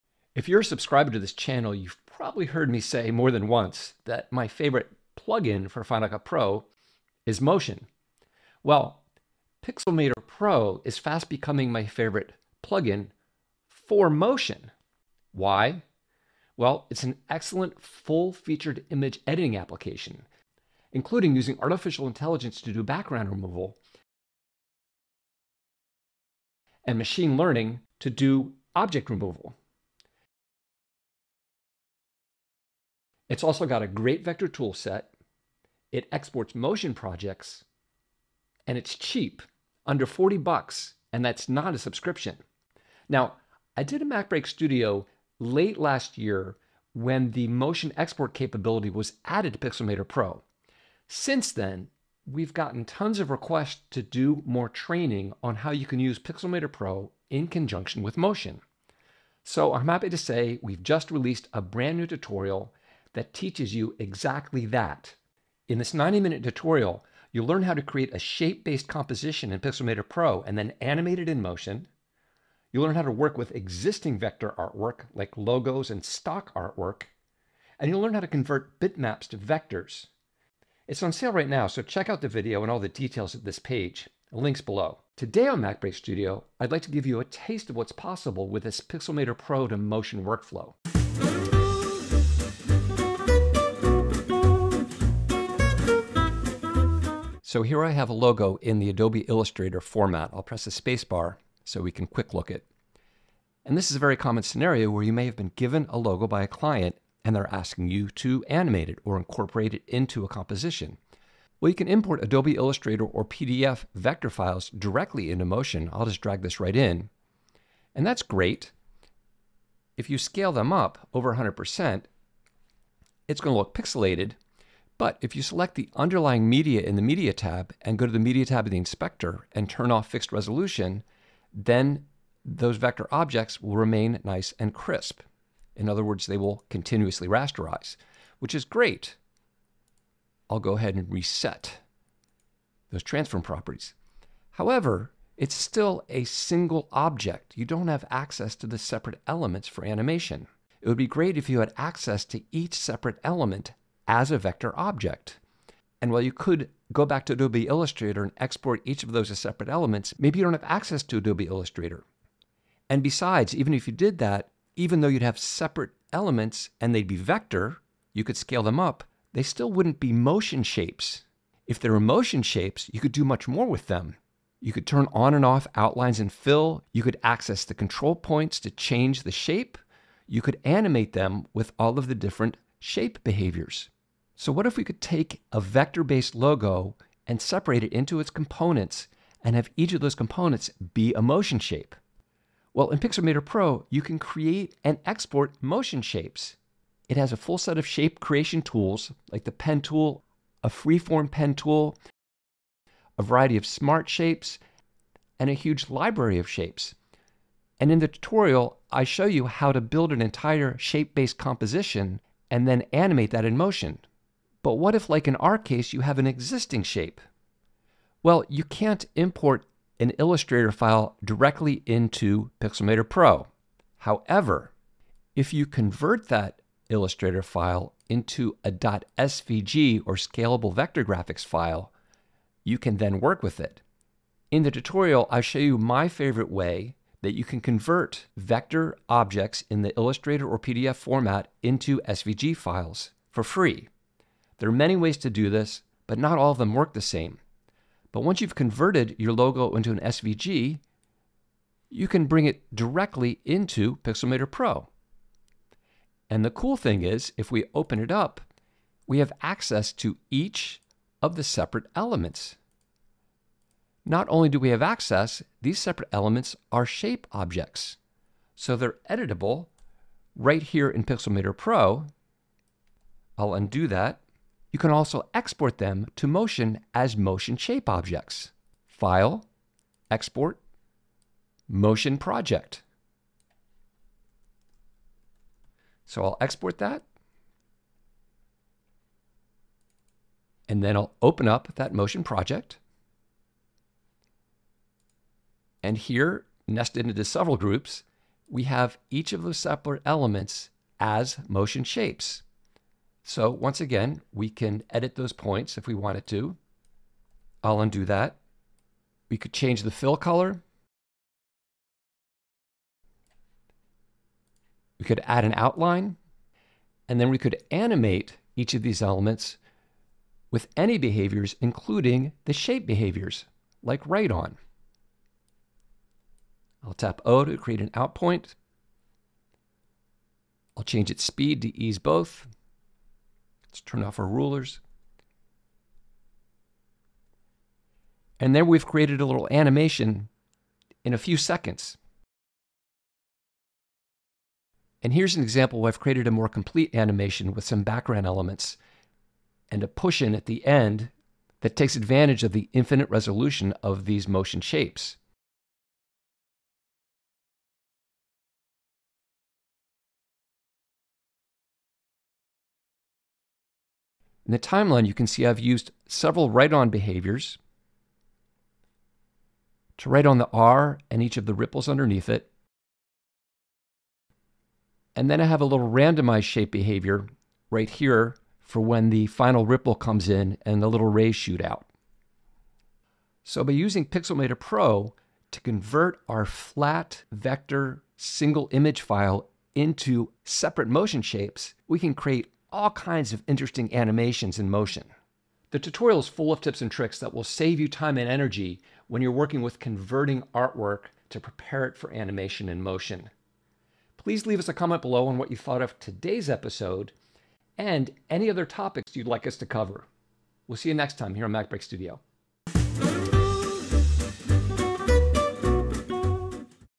Pixelmator Pro Vectors to Motion Shapes
Watch Mark demonstrate how he uses Pixelmator Pro to create vector graphics from a flattened logo then bring into Motion to animate the layers. Mark's new Pixelmator Pro to Motion tutorial

Use this code for discount - pixel-20

Chapter hyperlinks

:14 - Why use Pixelmator Pro?
1:04 - What's in Mark's New Tutorial?
2:23 - The Benefit of Exporting Motion Shapes